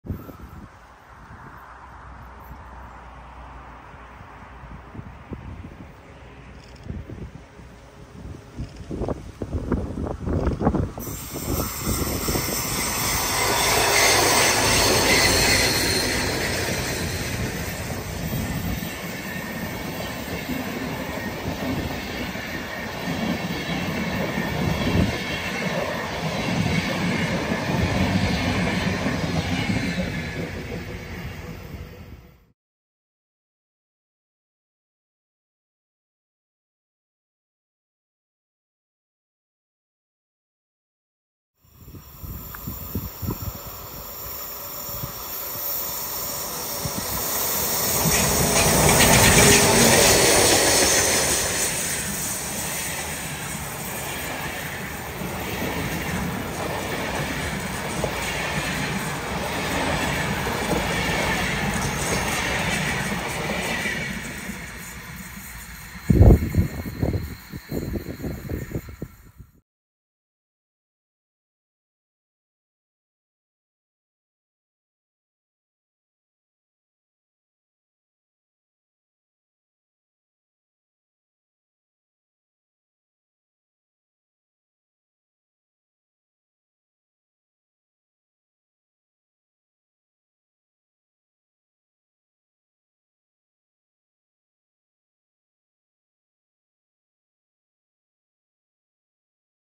IC 2012/13 im Ausnahmezustand auf der Geislinger Steige: mit BR101, Press218 und Steuerwagen!
Am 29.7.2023 kam IC2012 „Allgäu“ aus Oberstdorf die Steige hinab, an sich ein täglicher Anblick, aber die aushilfsweise im Allgäu weilende Press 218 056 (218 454) in blau ist schon etwa Besonderes.

Offenbar hatten aber beide 218 (die Press Lok sowie 218 424) technische Probleme, so dass direkt 218 446 nach Ulm eilte, und der IC 2013 dann von 101 013–1 geführt von Stuttgart bis Ulm fuhr!

So konnte man also 101 013, dann 218 056, dann 218 424 am IC 2013 die Steige hochgahre. sehen, als zusätzliche Überraschung hing ein Steuerwagen am Zugschluss!
Ab Ulm ging es dann mit drei verschiedenfarbigen 218 bis Oberstdorf!

On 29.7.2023, IC2012 "Allgäu" came from Oberstdorf down the Geislinger Steige, usually a daily sight, but Press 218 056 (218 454) in blue, which was temporarily in the Allgäu is something special.

Apparently, both 218s (the Press locomotive as well as 218 424) had technical problems, and the IC 2013 was then led by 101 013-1 from Stuttgart to Ulm!

So you could see 101 013, then 218 056, then 218 424 on the IC 2013 climbing up the Steige. As an additional surprise a control car was at the end of the train!
From Ulm it then went with three different colored 218 to Oberstdorf!